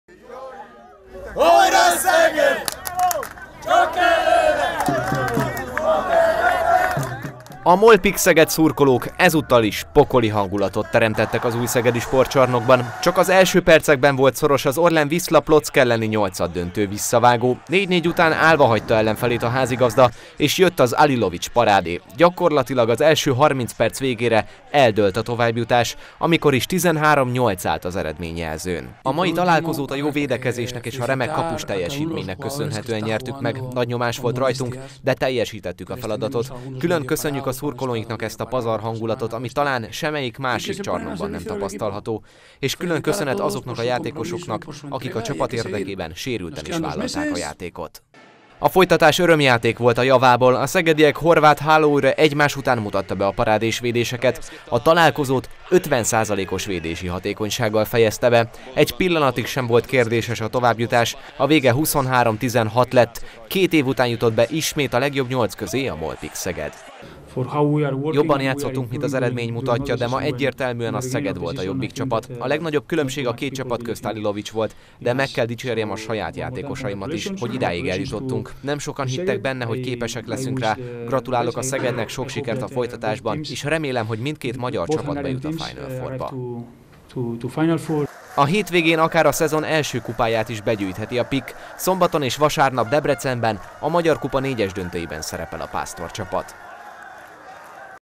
Újabb lépés Köln felé - Szegedi Hírek - 19.04.01.
Legjobb nyolc között a MOL-Pick Szeged. A Bajnokok Ligája nyolcaddöntőjének visszavágóján 23–16-ra legyőzte a magyar bajnok a lengyel Orlen Wisła Płockot, így kettős győzelemmel jutott tovább a gárda. A találkozó hőse az a Mirko Alilovic volt, aki ötven százalékkal védett a meccsen.